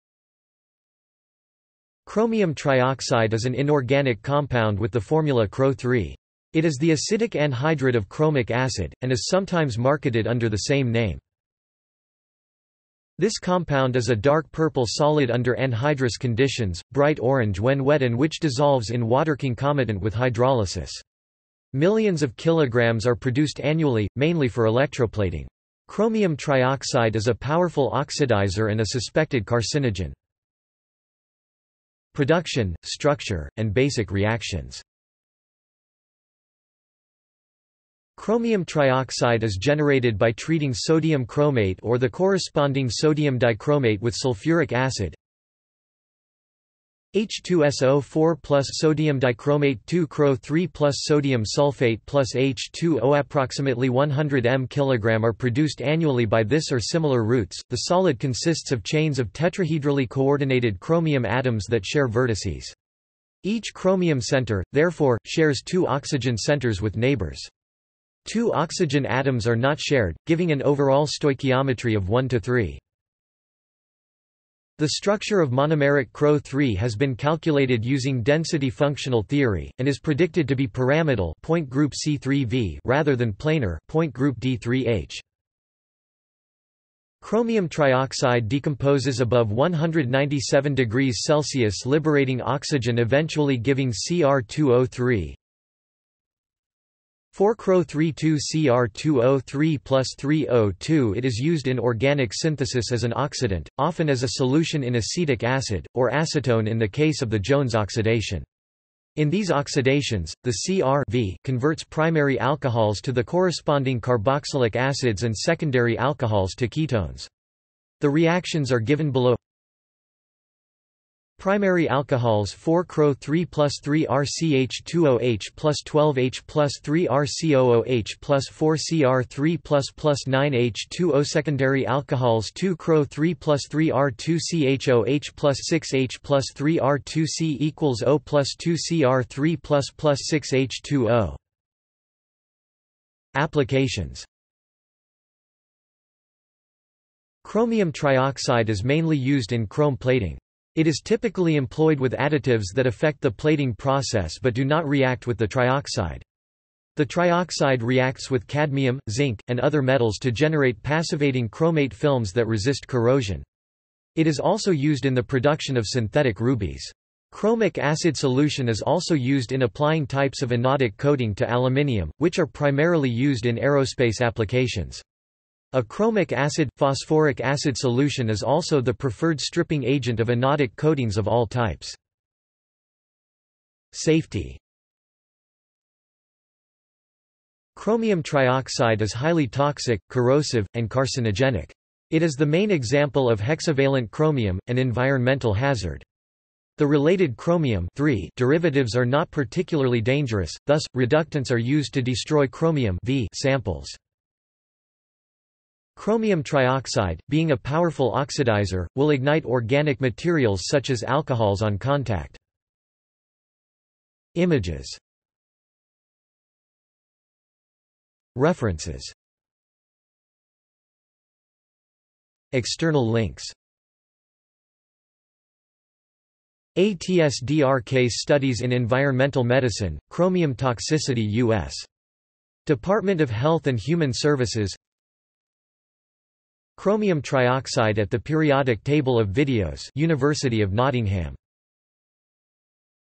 Chromium trioxide | Wikipedia audio article
This is an audio version of the Wikipedia Article:
Chromium trioxide

SUMMARY
Chromium trioxide is an inorganic compound with the formula CrO3.  It is the acidic anhydride of chromic acid, and is sometimes marketed under the same name.
This compound is a dark-purple solid under anhydrous conditions, bright orange when wet and which dissolves in water concomitant with hydrolysis.  Millions of kilograms are produced annually, mainly for electroplating. Chromium trioxide is a powerful oxidiser and a suspected carcinogen.